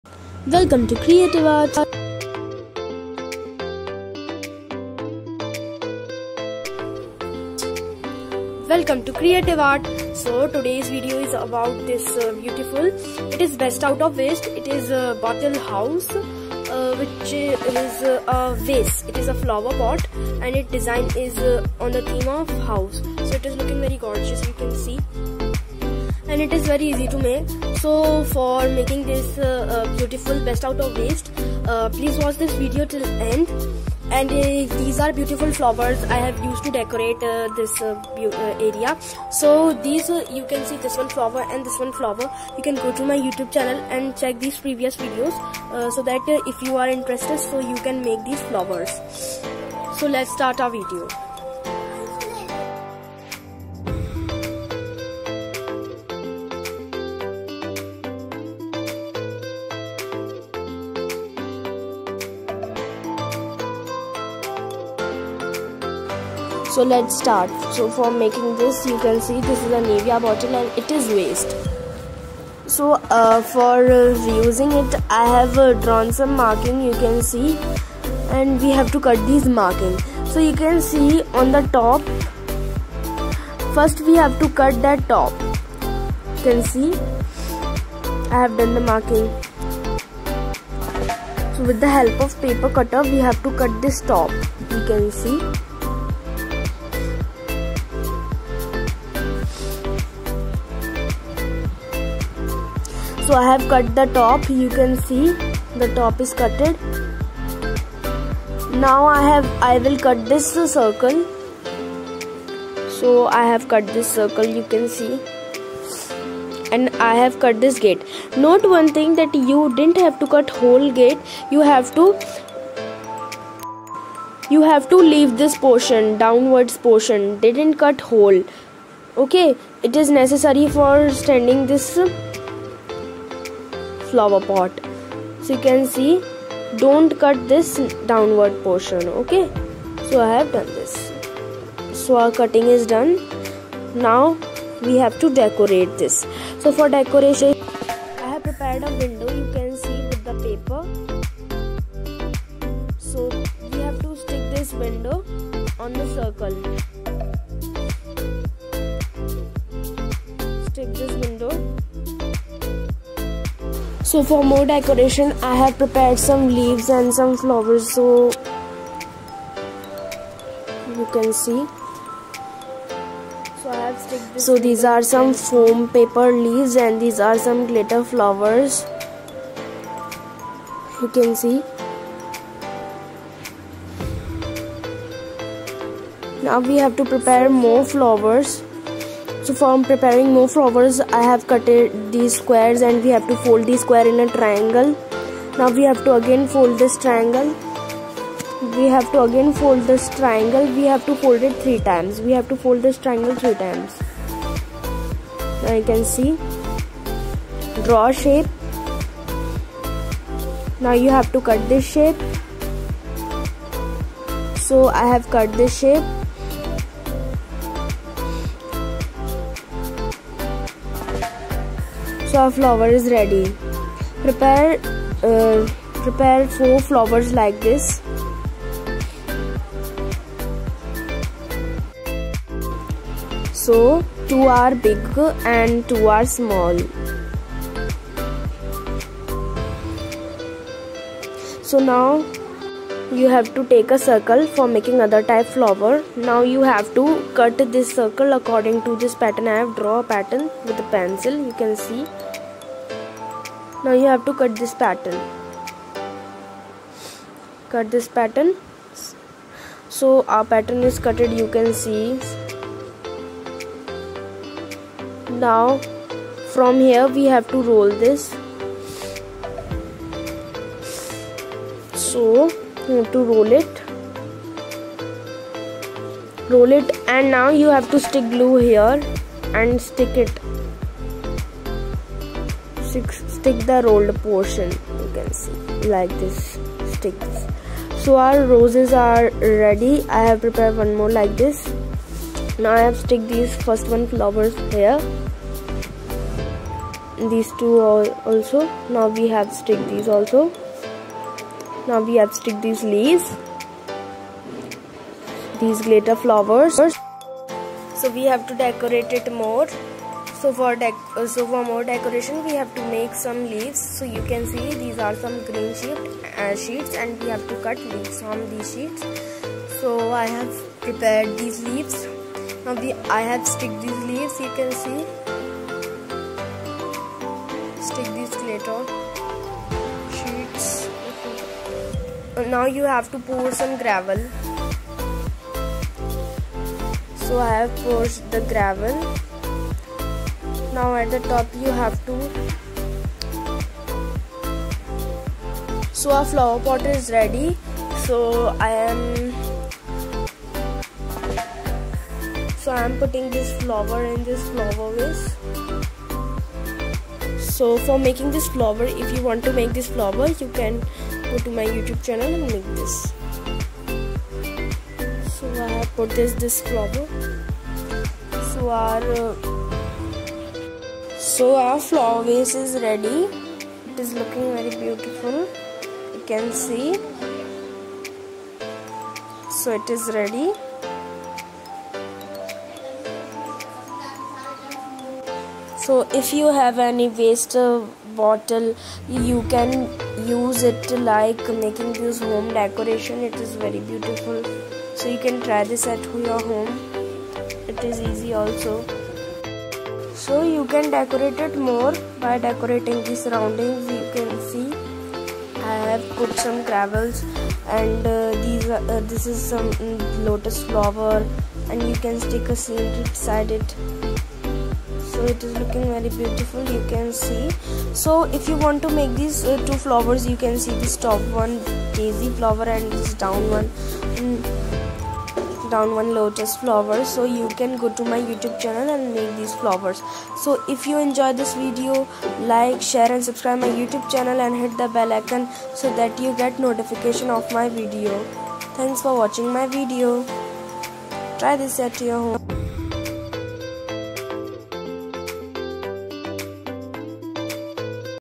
Best out of waste( house )flower pot by( body lotion) bottle easy don't throw waste bottle
If you have any waste bottle of body lotion don't throw it  make use of it by making this beautiful  flower pot best out of waste try this at home .